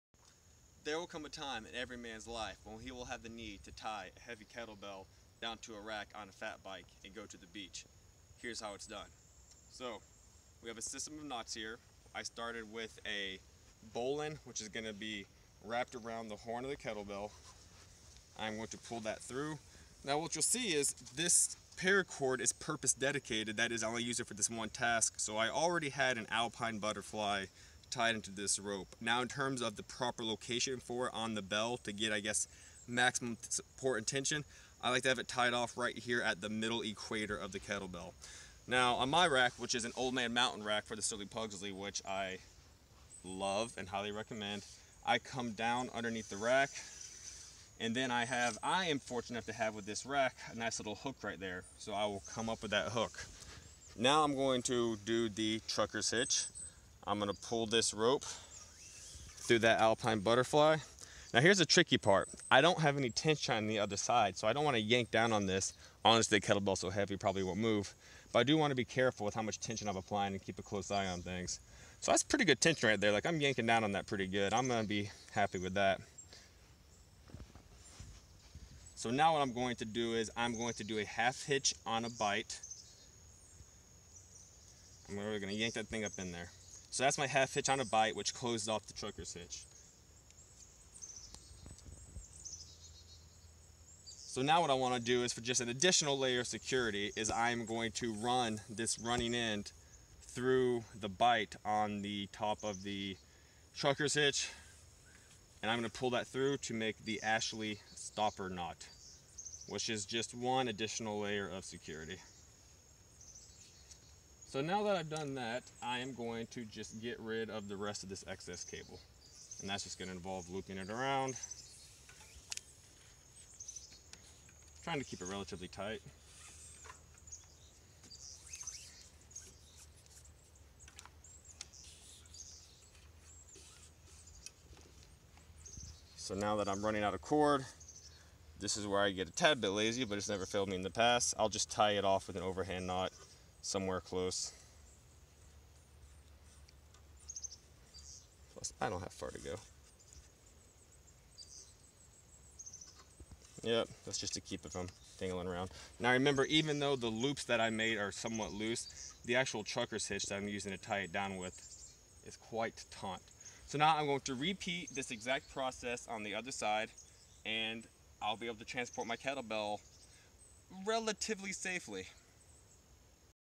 How I: Tie a kettlebell to a bike for transport
I often find myself ~needing~ to transport my kettlebell via bike. Here is how I do it.

This is a "How I" video versus "How to." "How to" videos suggest a course of action is coming from an expert, which I am not. Therefore this is simply "How I" do things, proceed with caution.